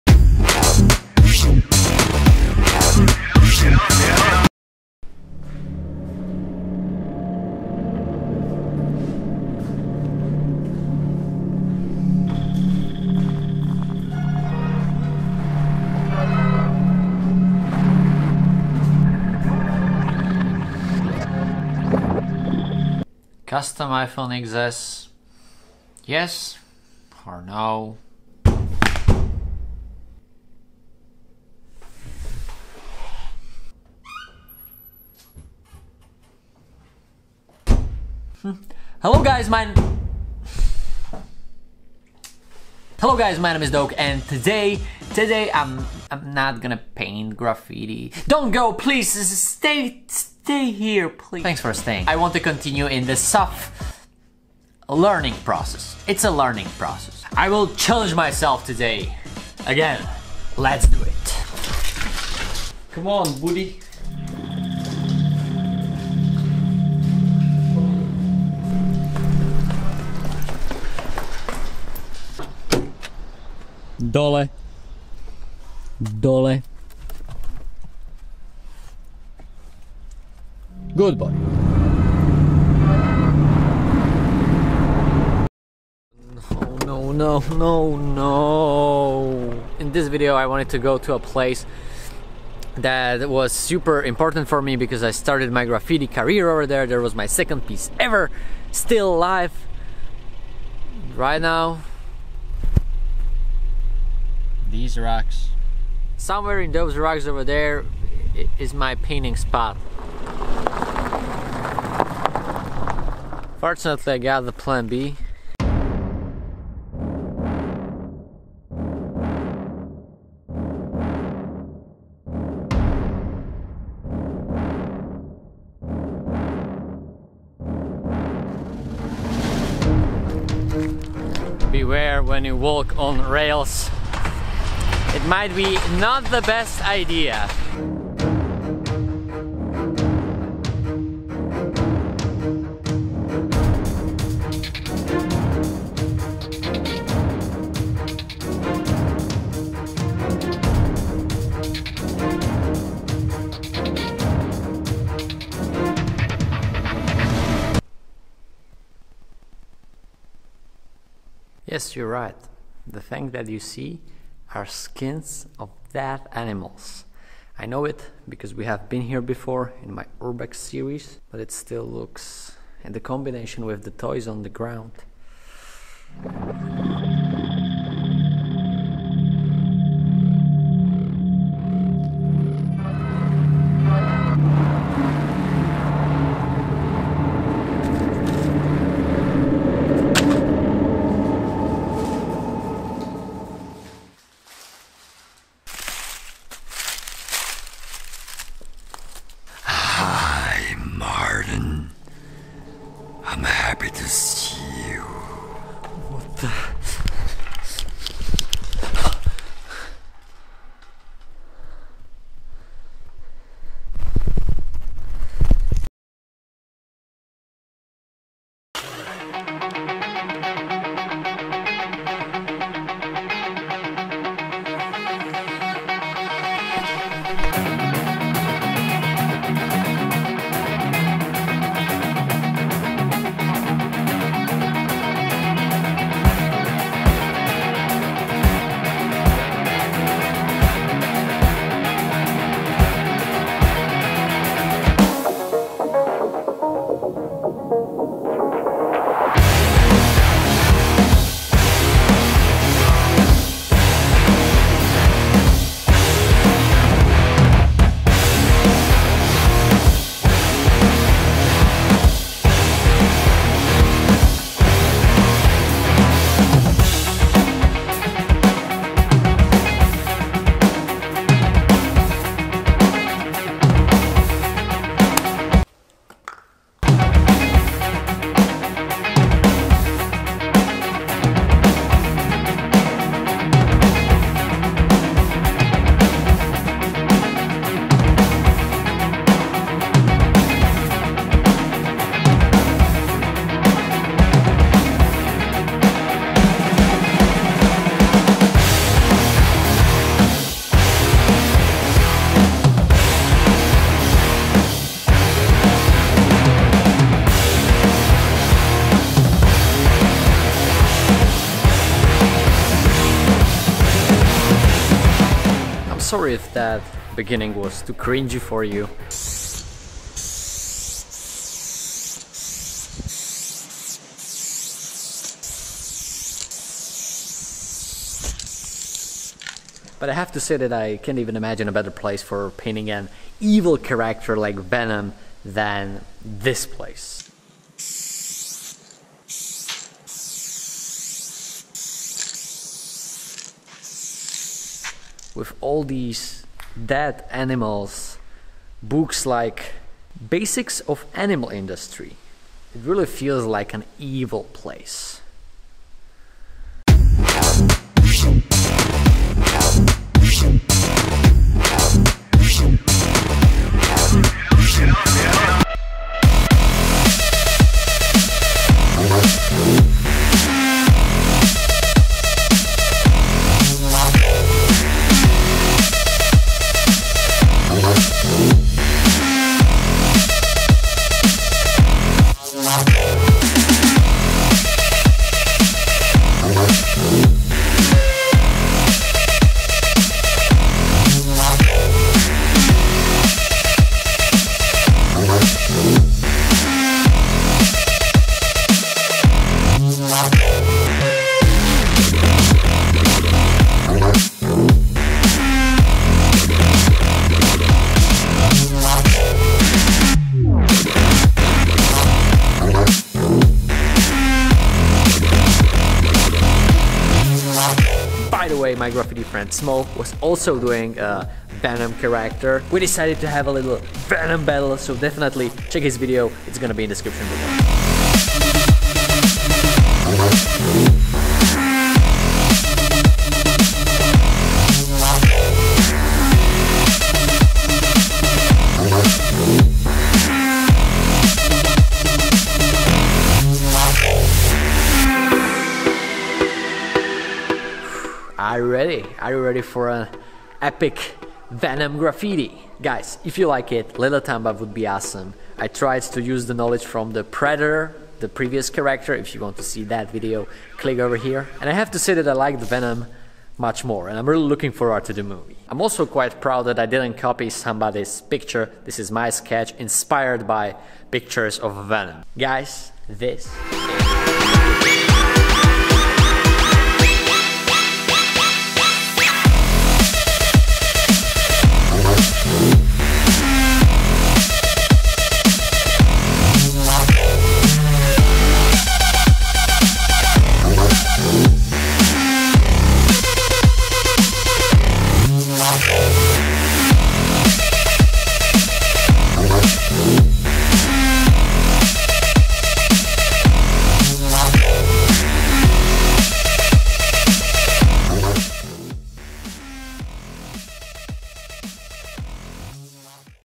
EPIC VENOM Painting!
Epic Venom Graffiti painting in abandoned building. I have seen the Venom trailer and I got inspired to create this painting.  SMOEs VIDEO

SEND ME SOMETHING:

Martin Hirner
P.O.BOX 12
85003, Bratislava 53
Slovakia